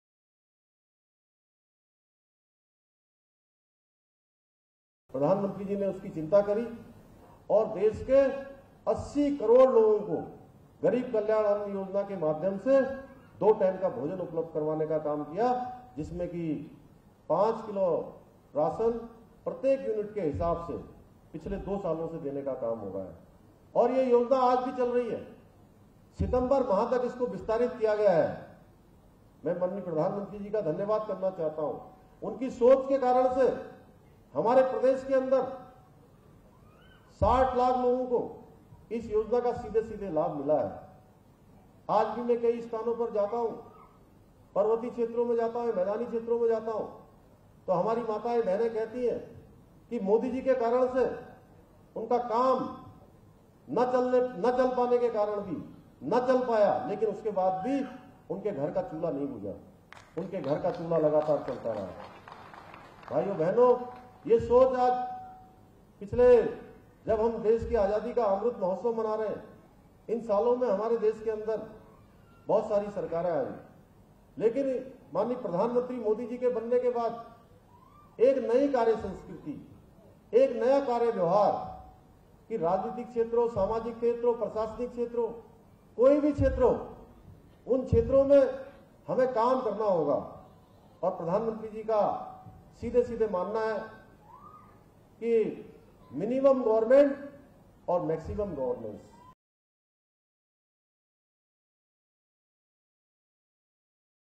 Uttarakhand CM Pushkar Singh Dhami virtually attends PM Modi’s ‘Garib Kalyan Sammelan’
Panthnagar (Uttarakhand), May 31 (ANI): At the ‘Garib Kalyan Sammelan’ event, Uttarakhand Chief Minister Pushkar Singh Dhami on May 31 addressed the beneficiaries of the scheme. “Under PM Garib Kalyan Anna Yojana, about 60 lakh people of the state have been benefitted, with the poor availing meals twice a day. The scheme has been extended till September...Such schemes reflect minimum government and maximum governance,” said CM Dhami. (HR)